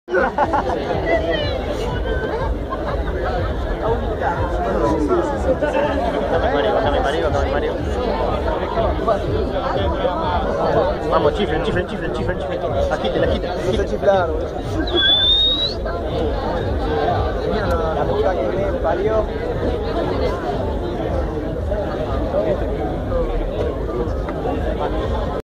CREAMFIELDS BA 2015 - 15th Aniversary & My 35th Birthday - VIDEO 11
Ingresando al Predio de CREAMFIELDS BA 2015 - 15th Aniversary & My 35th Birthday